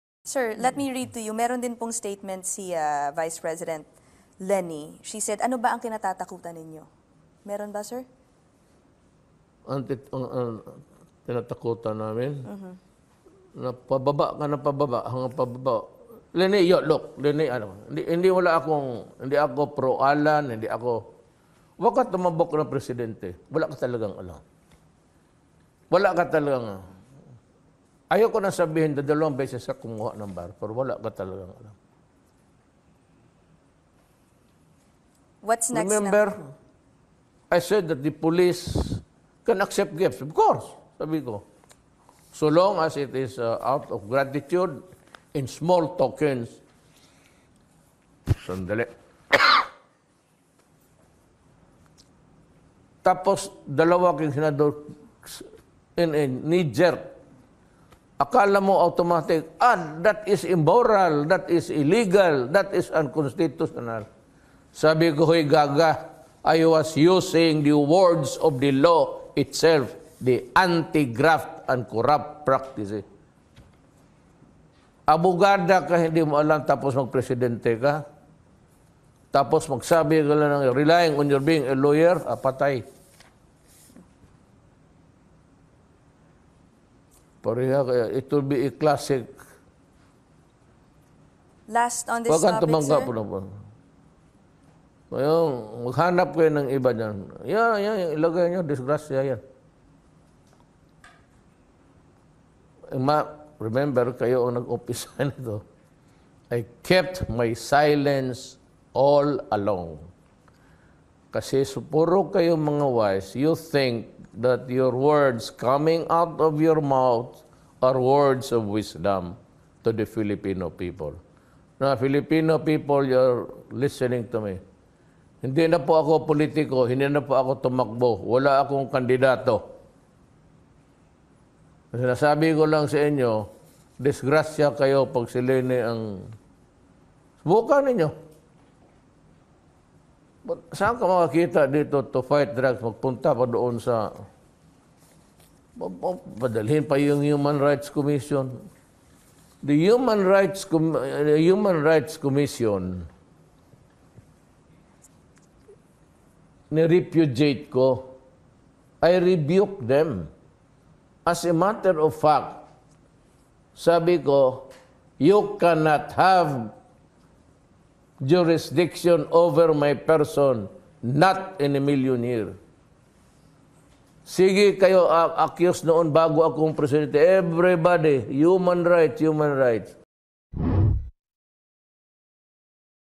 President Duterte to VP Leni
Panayam ky PRRD tungkol s pgtakbo ni VP Leni.
Disgrasya nga b? Kuo n po ang Humusga mga k sweet...